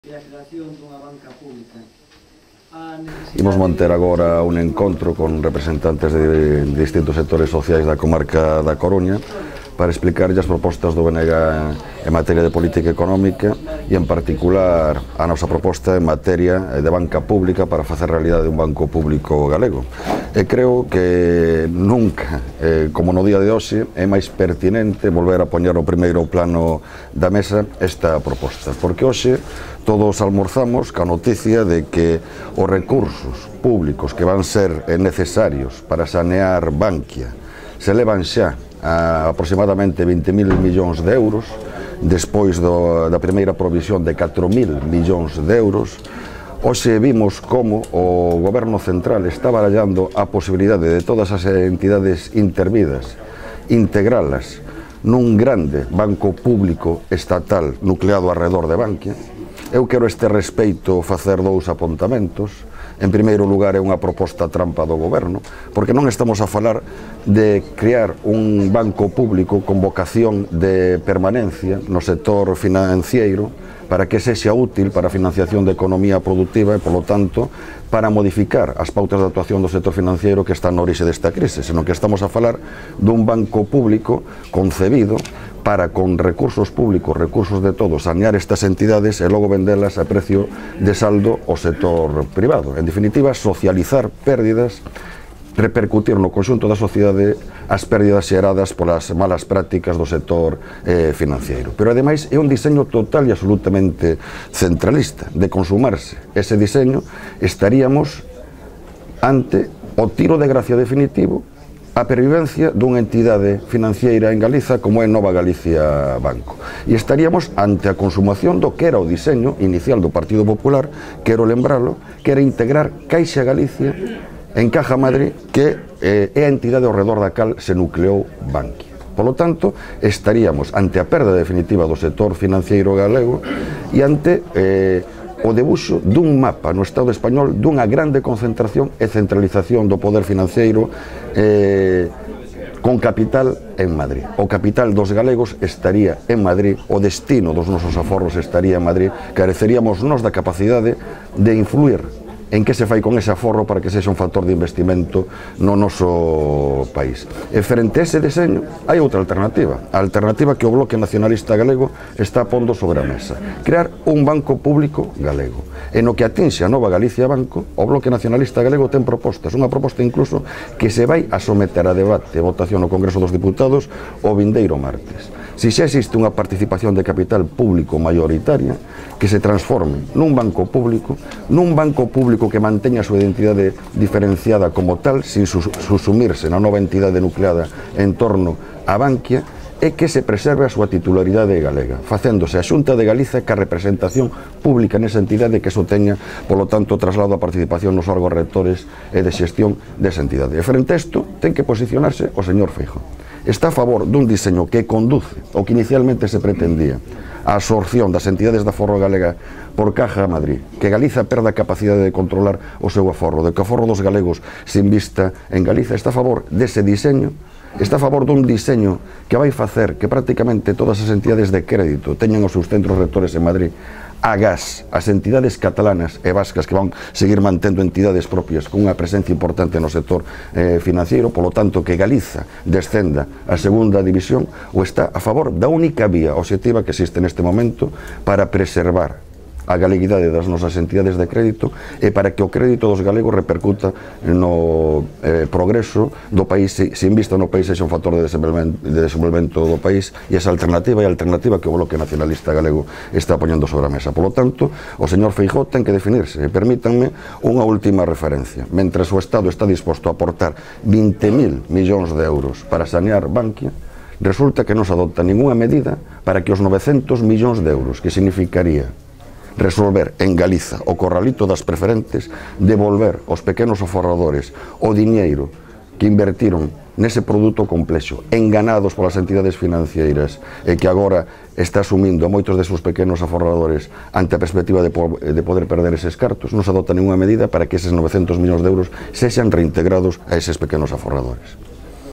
Jorquera a Feijoo: "Ou banca pública galega ou absorción de Novagalicia Banco por Bankia"
O candidato do BNG á presidencia da Xunta de Galiza, Francisco Jorquera, instou hoxe a Núñez Feijoo a que tome partido pola Banca Pública Galega e non pola absorción de Novagalicia Banco por Bankia, tal e como pretende o Goberno Central.

Jorquera interveu esta mañá na Coruña nun acto para presentar a proposta do BNG de crear unha Banca Pública Galega. Canda el estiveron o catedrático de Estrutura Económica da USC, Xavier Vence, e o portavoz municipal do BNG, Xosé Manuel Carril, que presentaron a proposta a representantes de entidades sociais e económicas da Comarca como como os sindicatos CIG e CCOO, do sector do pequeno comercio, de profesionais autónomos, asociacións de veciños ou representantes empresariais.